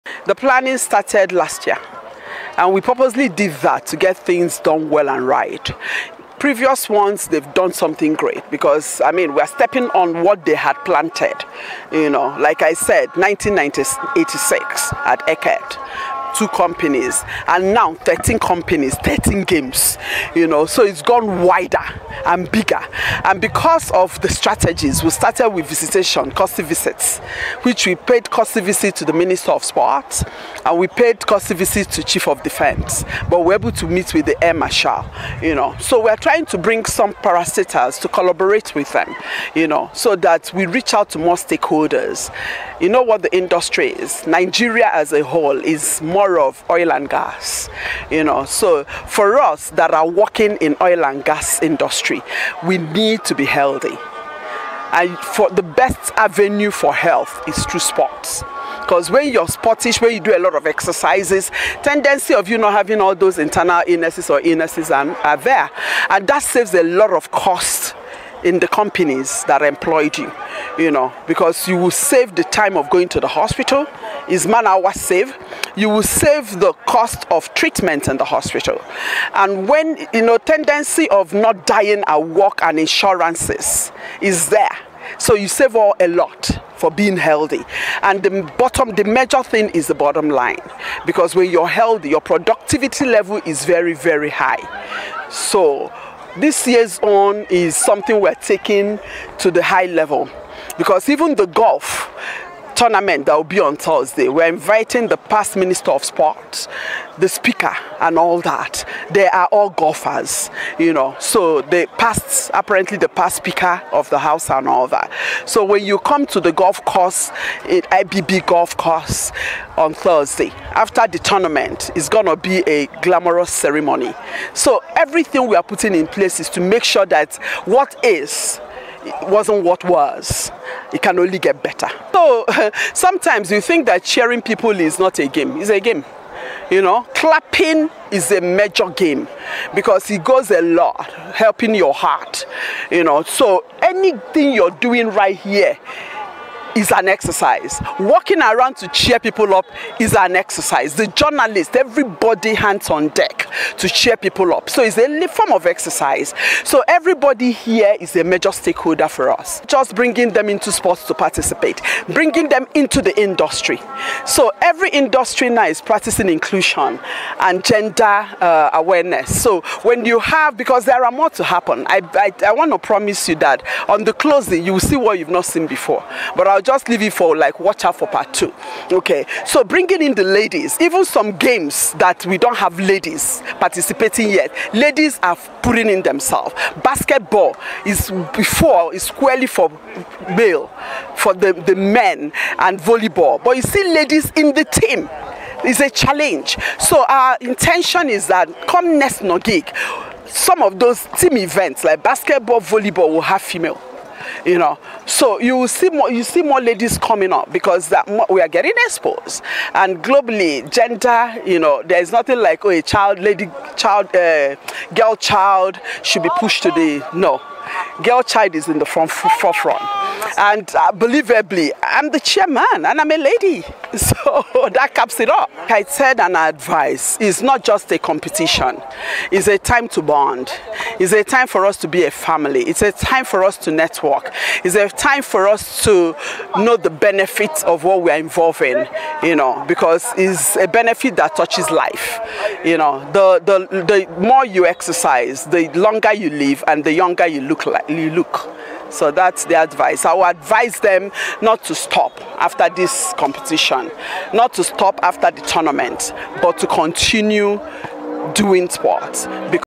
Dr. Ifeyinea Mojo-Eyes , Chairman LOC NOGIG Charged Members To Always Engage In Sporting Activities
BOJ TV NIGERIA